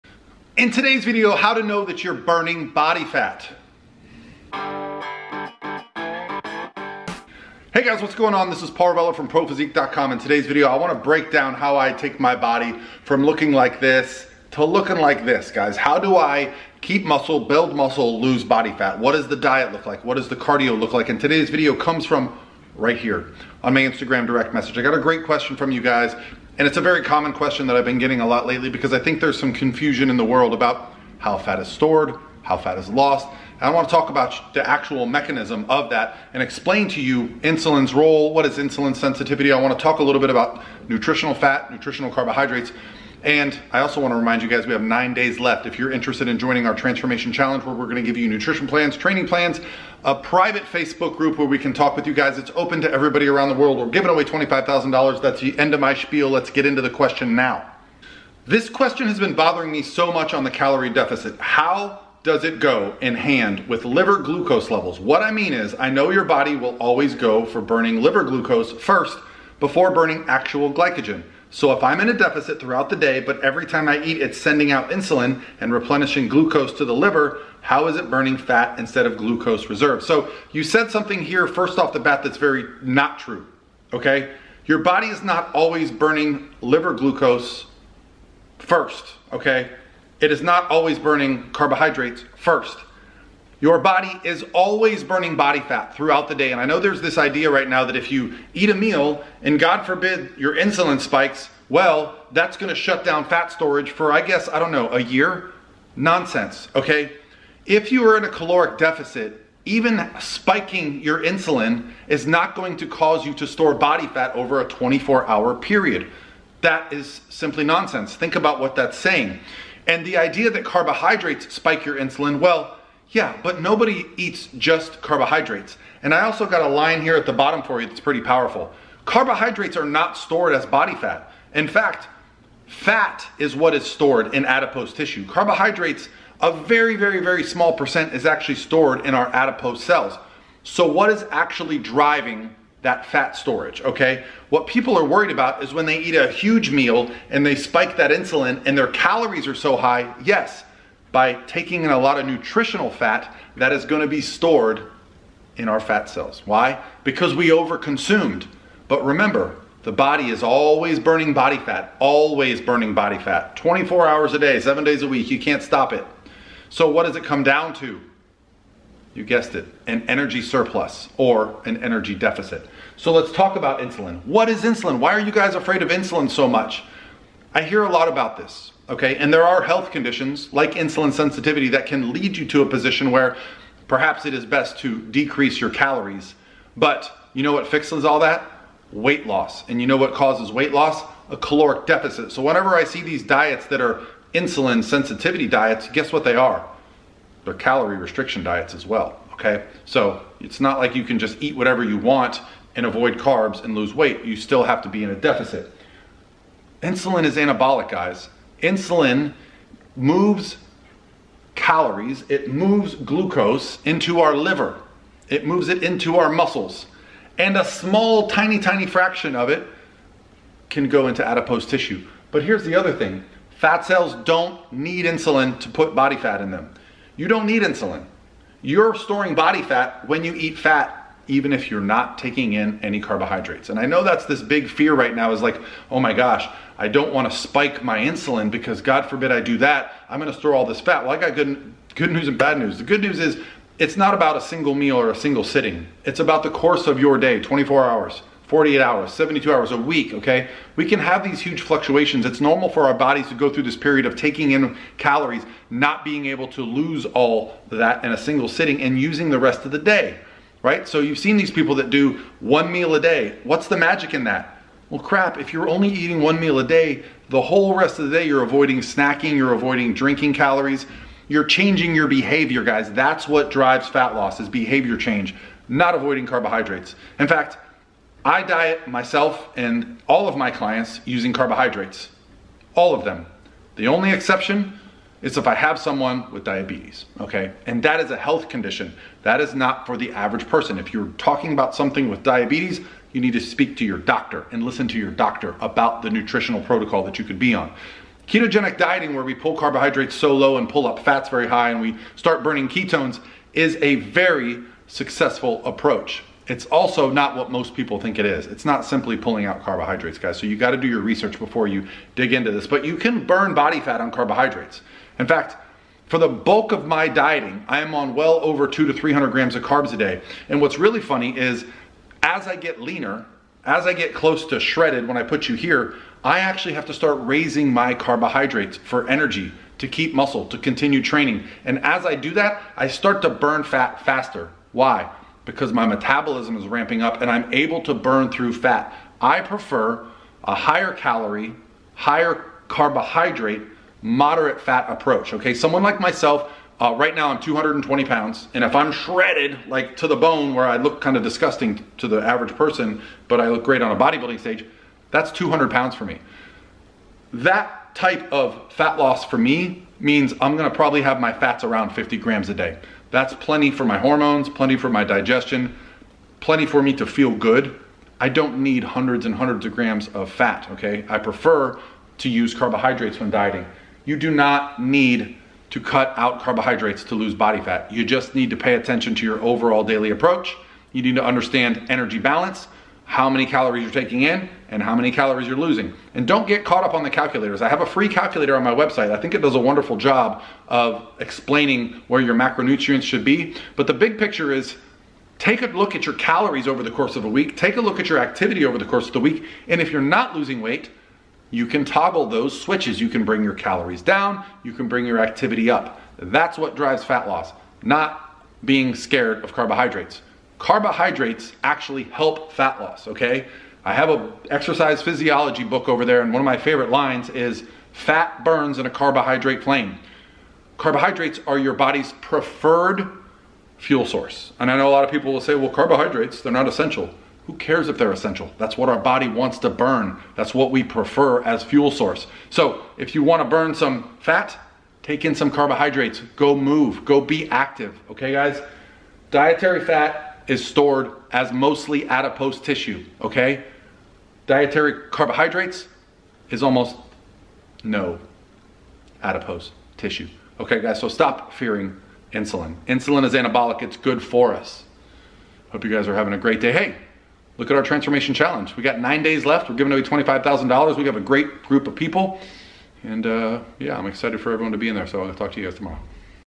How To Know You Are Burning Body Fat
To get rid of body fat on your lower back, you’ll need to start by creating a caloric deficit. That means that you’ll need to burn more calories than you consume.

In addition to cutting calories, you can tone your back muscles if you focus your exercise routine to target the muscles in your upper and lower back.

Insulin helps the body store fat, and this body fat that you store comes from the excess in calories. Insulin is also helpful in building muscle so it's role is passive, it responds based on your lifestyle.

how do you know that your body is burning body fat?

How do you lose lower back fat?